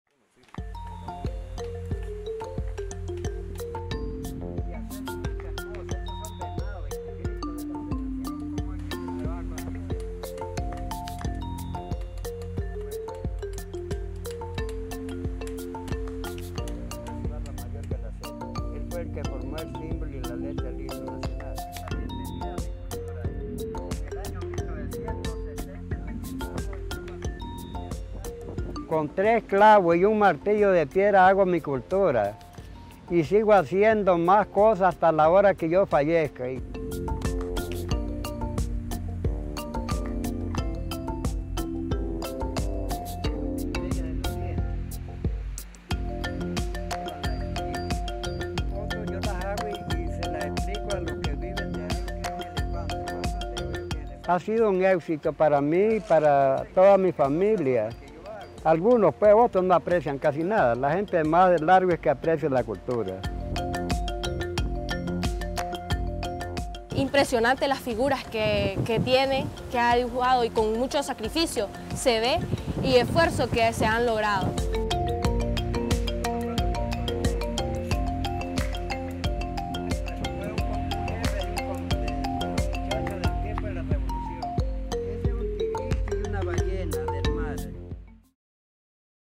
Mann verwandelt Berg Nicaraguas in Kunstwerk
Seit vier Jahrzehnten hämmert, klopft und malt dieser 78-Jährige mit drei Nägeln und einem Steinhammer am Gestein eines Berges in Nicaragua.